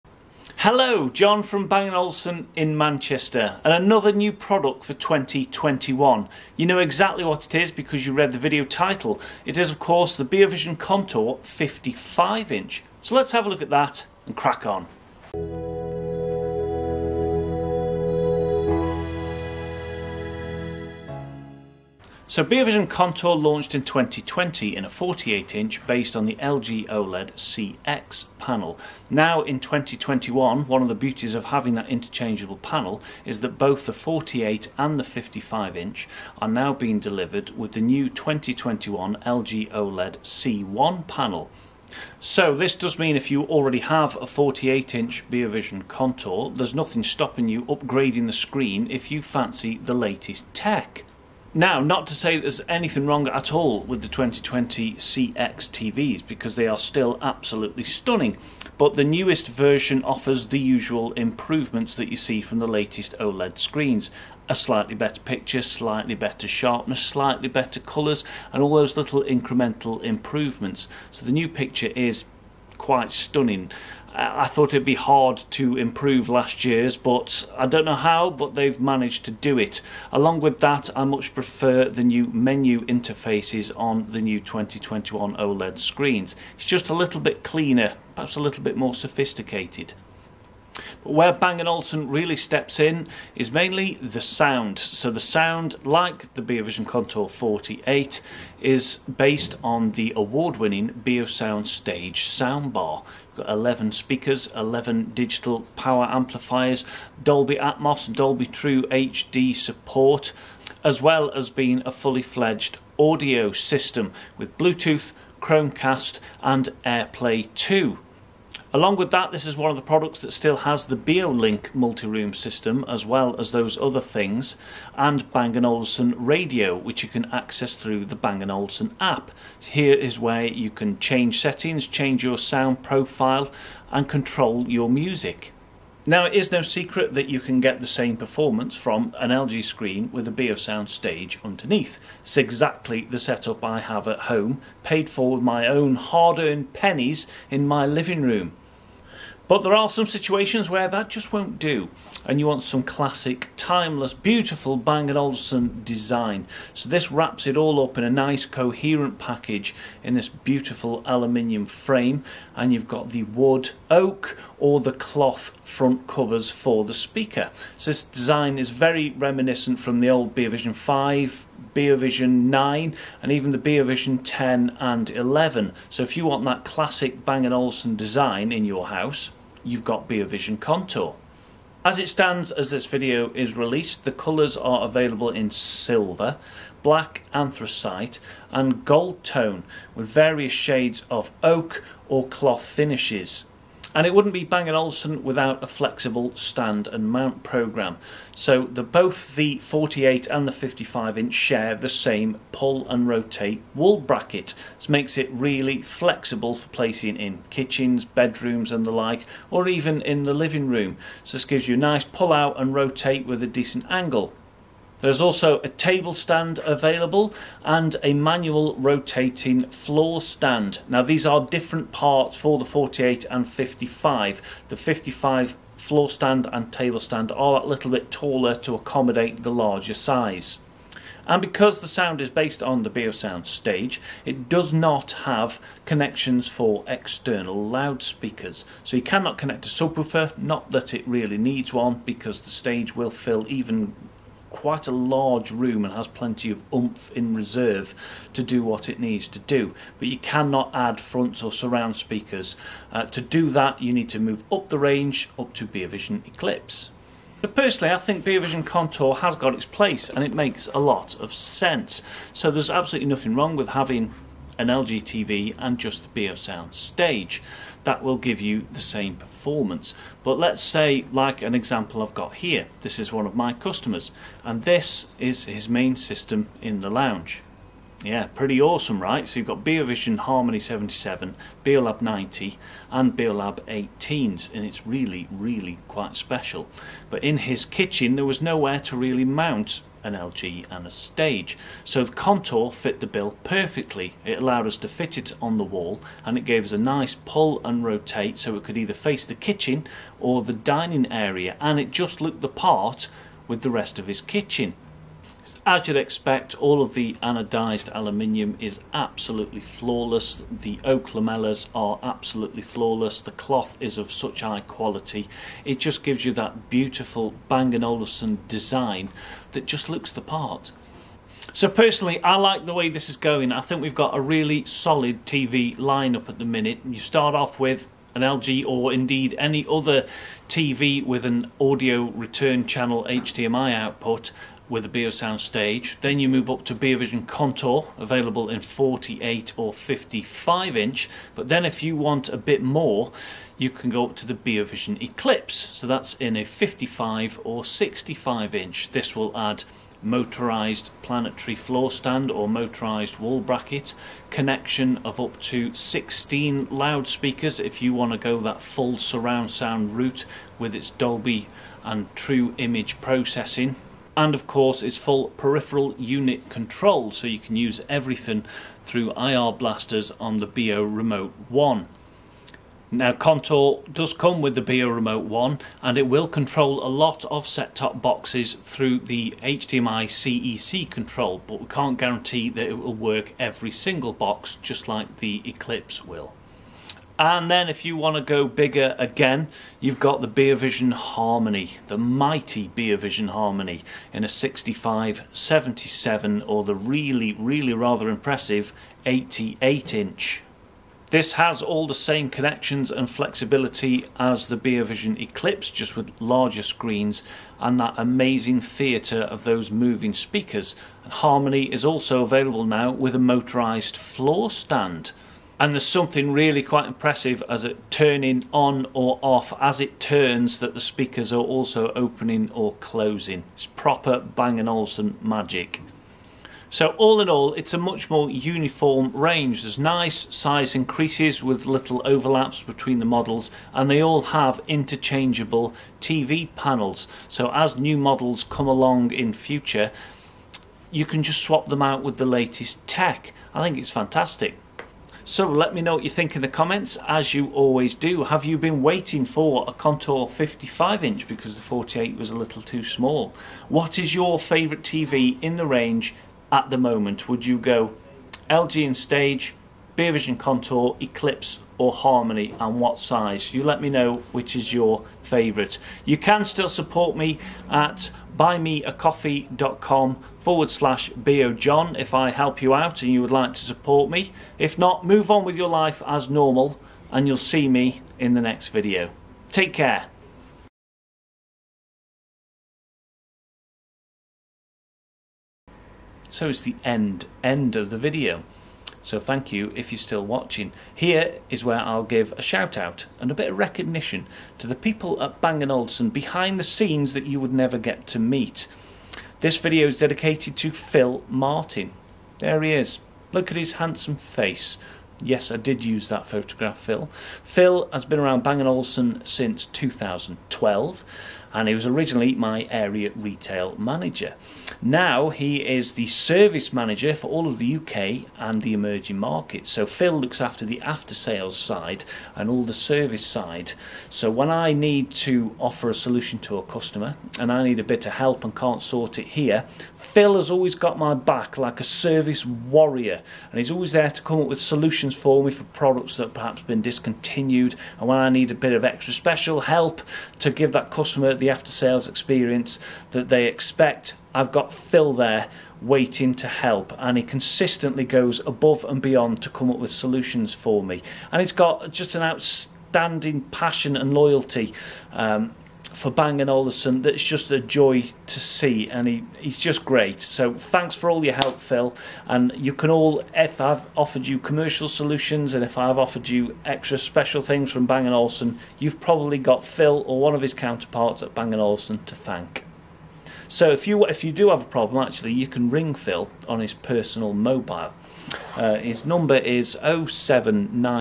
Beovision Contour 55 / 2021 Bang & Olufsen TV Line Up
Beovision Contour 48" and now 55" plus a rundown of the complete Bang & Olufsen TV range for 2021 with Beovision Eclipse Gen 2 and Beovision Harmony